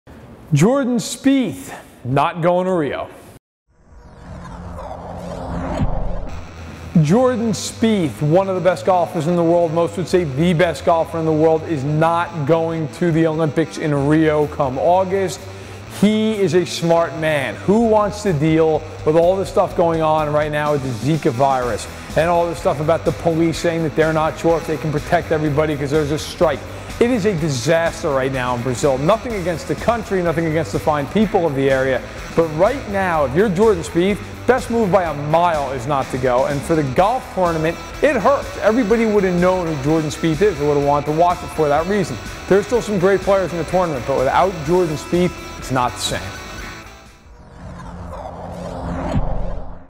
Jordan Spieth Back Out Of Rio 2016 Olympics
Pro golfer Jordan Spieth has backed out of playing golf for Team U.S.A. at the Rio 2016 Olympics.